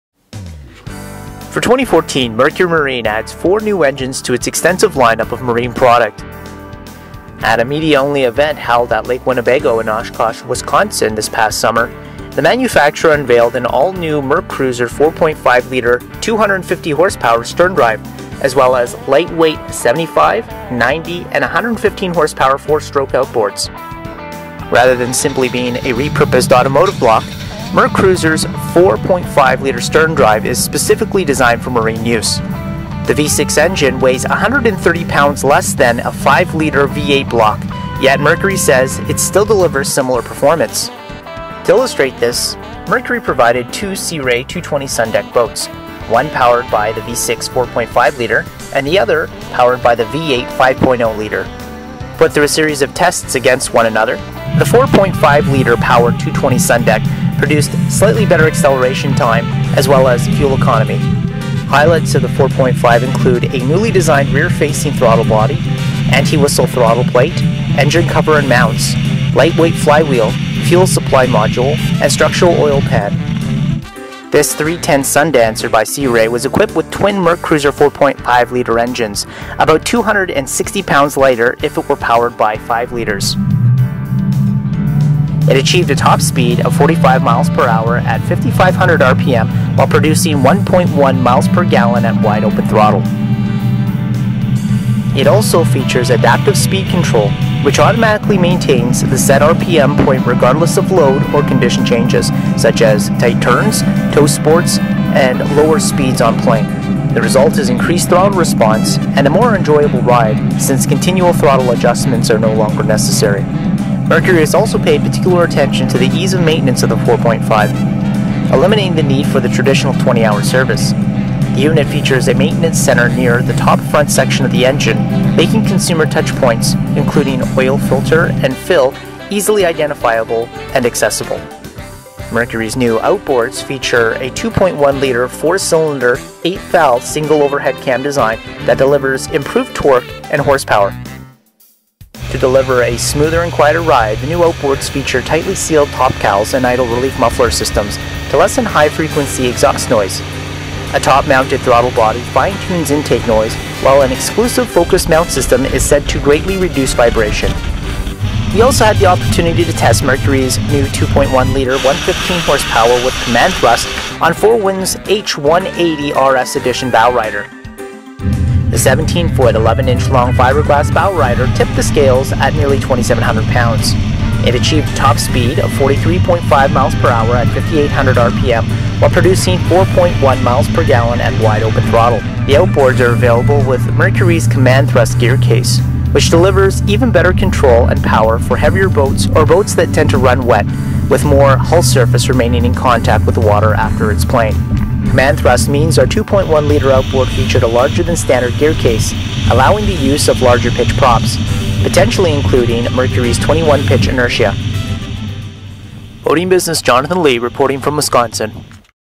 New Mercury 2.1L Outboards and MerCruiser 4.5L Sterndrive
At a media-only event held in Oshkosh, Wisconsin in June, Mercury unveiled its all-new MerCruiser 4.5L 250hp V6 sterndrive as well as new lighter weight 75, 90 and 115hp four-stroke outboards. Approximately 30 journalists from around the world were present for the marine engine manufacturer's new product launch.